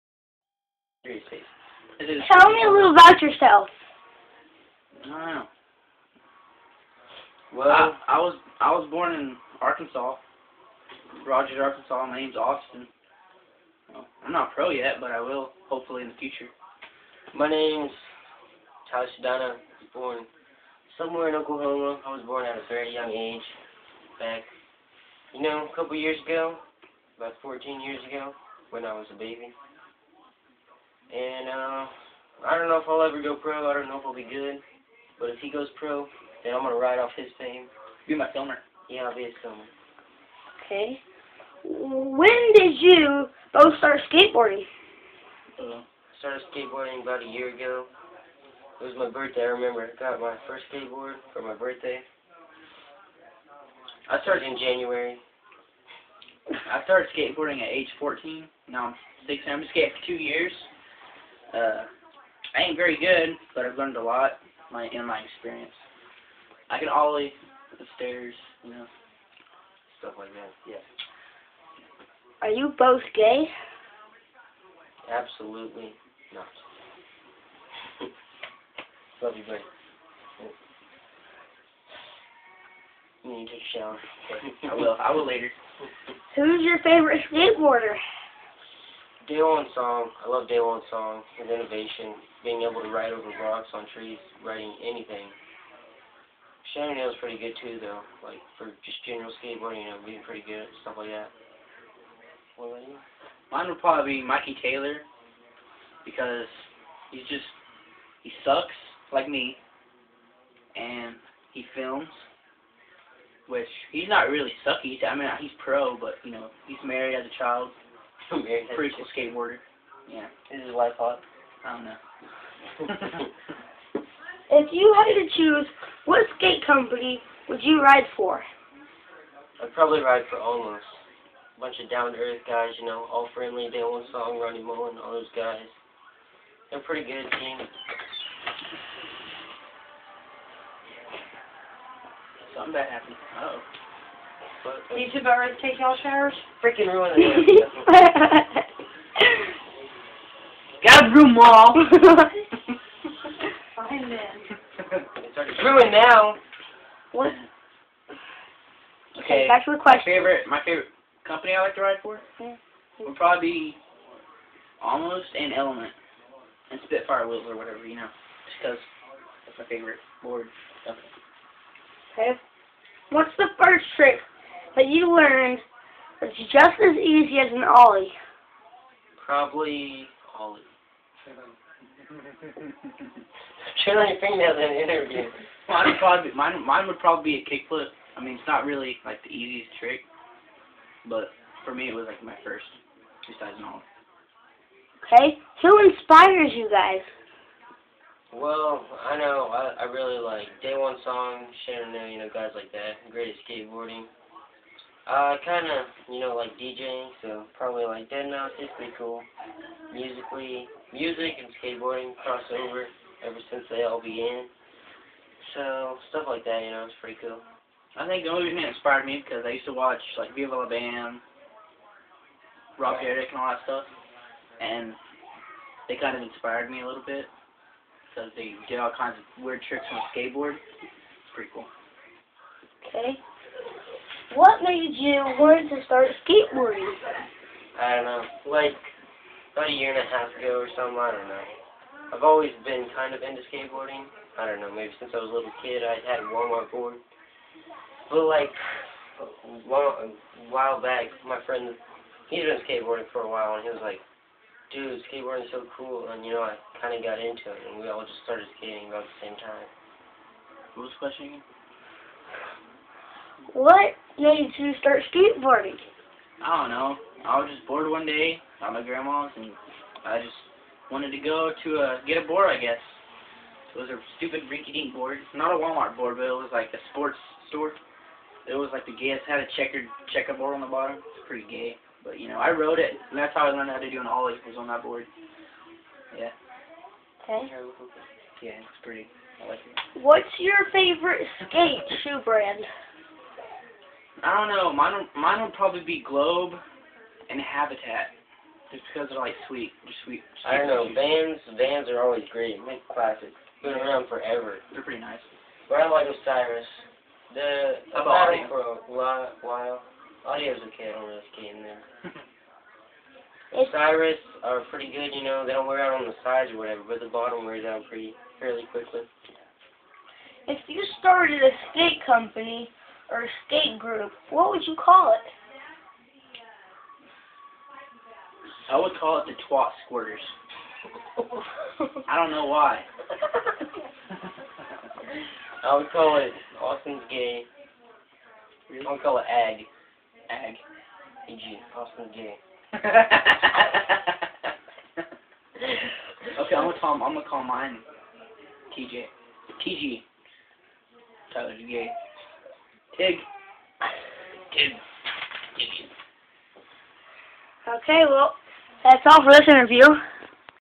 Tyler and austin interview
Austin james alverson and tyler sudano talk about when they first started skateboarding.